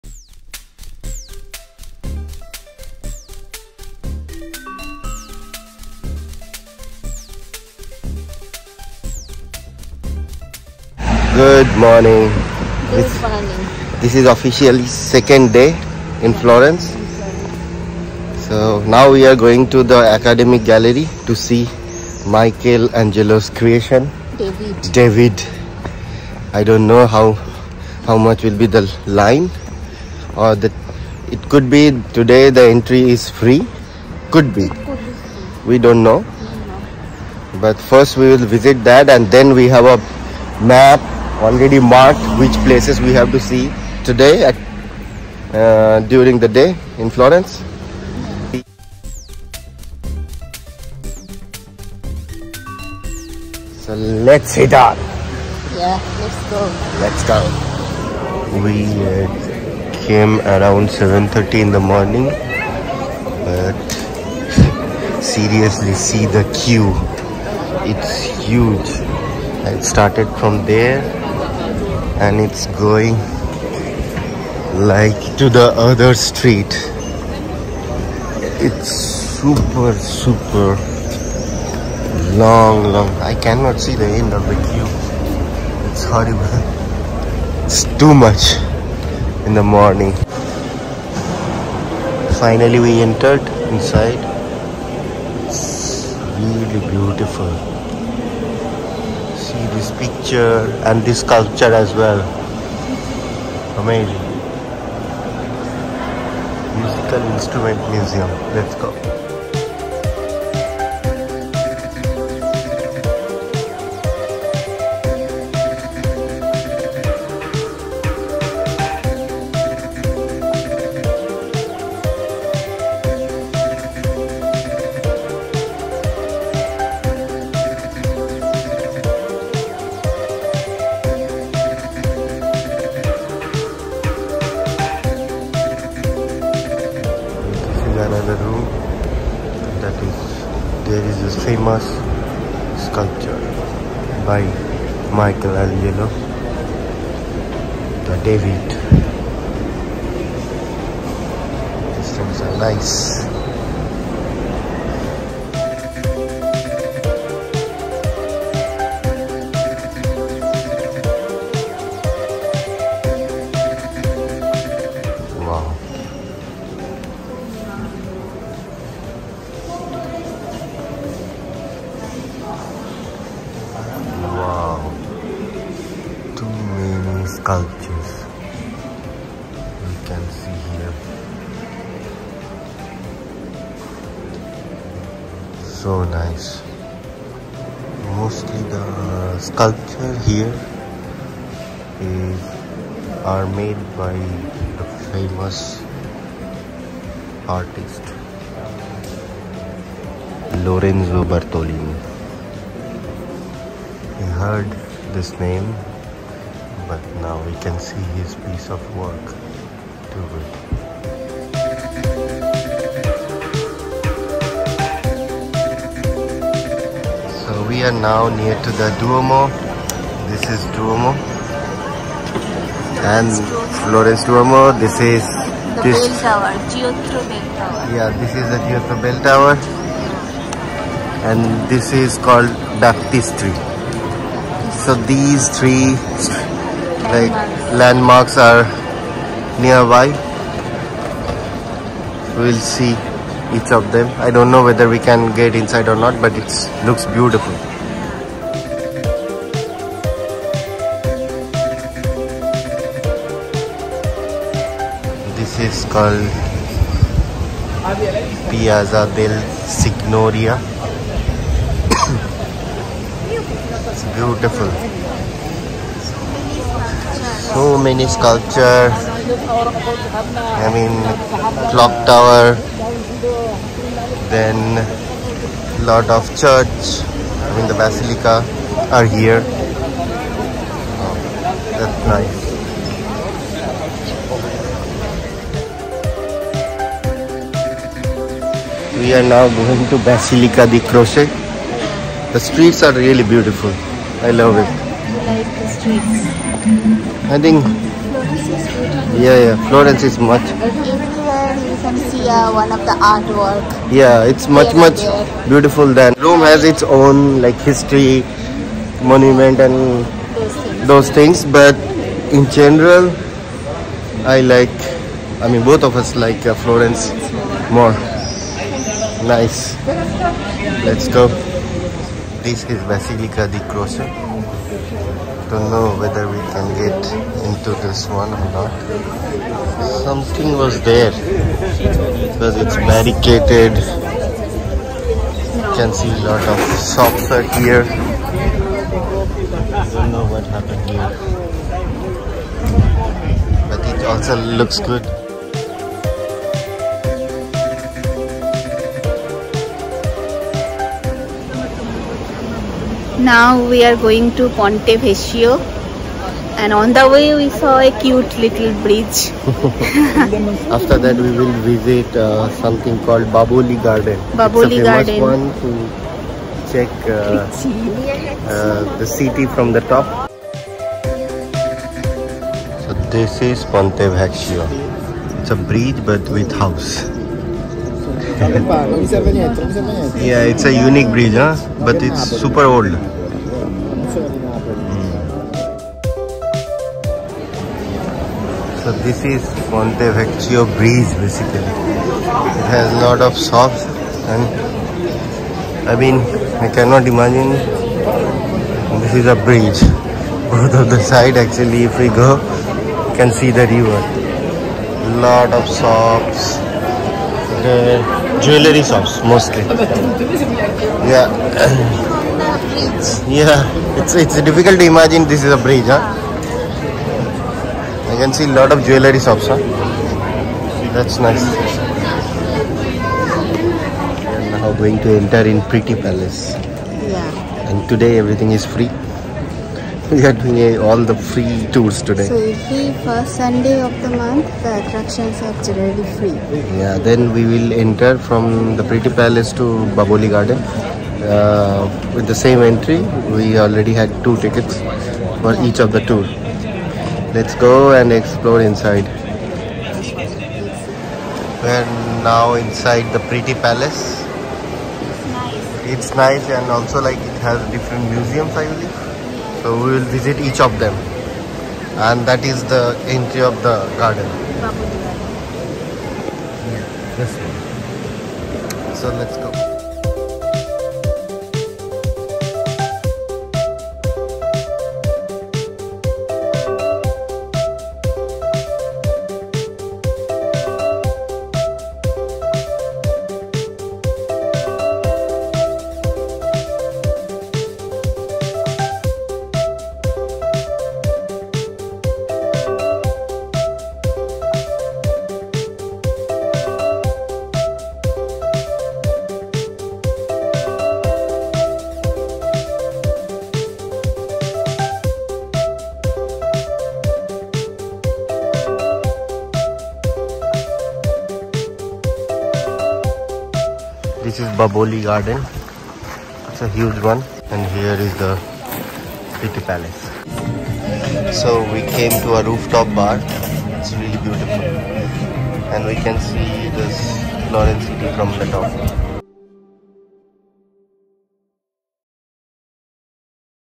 Italy Vlogs 2023 | Part 4: A Day in Florence | Vlog #58
In Florence, we spent our day visiting several attractions such as the Academia Gallery, Florence's Duomo, Giotto Bell Tower, Palazzo Vecchio, Piazza della Signoria, Piazza Santa Croce, Pitti Palace, and Ponte Vecchio.